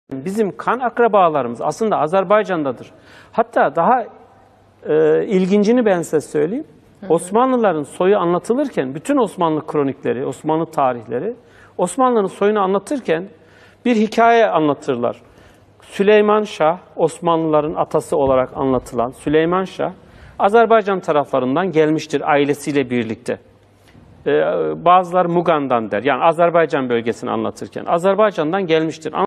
امپراتوری عثمانی اصالتا از جغرافیای آذربایجان و امپراتوری صفوی از جغرافیای ترکیه امروزی برخواسته بود
امپراتوری عثمانی رو میشه یجورایی آذربایجانی دونست! (سلیمان شاه جد  عثمان اول که پسر ارطغرل بود «سریال های دیریلیش ارطغرل و کورولوش عثمان رو دیدین!؟» از آذربایجان و سرحد دشت مغان [استان اردبیل] به آناتولی مهاجرت کرده بودن)

Osmanli devletin kuranlar, azerbaycandan ve mogan bolgesinden (bu gunki Ardabil serheddi) anadoluya getmişler
اما وقتی ماجرا جالب تر میشه که می‌فهمیم  موسسان سلسله صفویه که قزلباش ها باشن  از آناتولی که میشه ترکیه امروزی به آذربایجان اومده بودن  و امپراتوری صفوی رو پایه گذاری کردن!
Ve elbet, safavi devletin kuranlar, gizilbaş turkleri bu günki turkiyeden ( dogu Anadolu) azerbaycana gelmişler